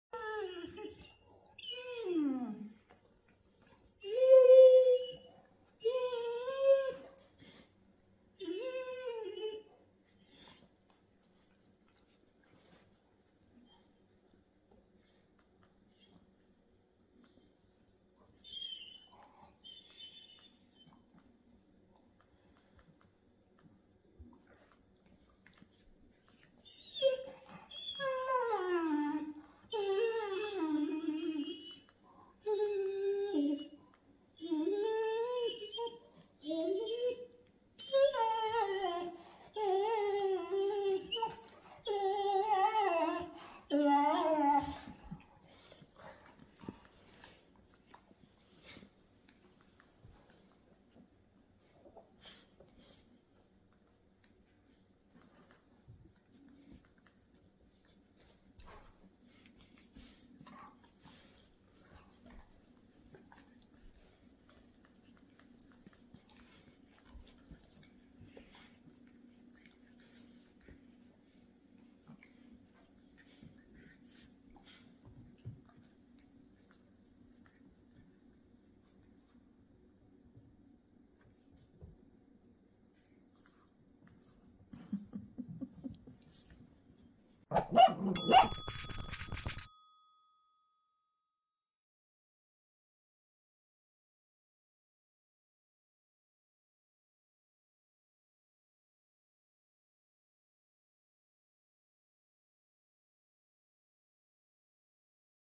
Boston Terrier Puppy Takes On Great Danes
7 week old Busby takes on his 7 month old brother Cooper as their big brother Kevin watches over them.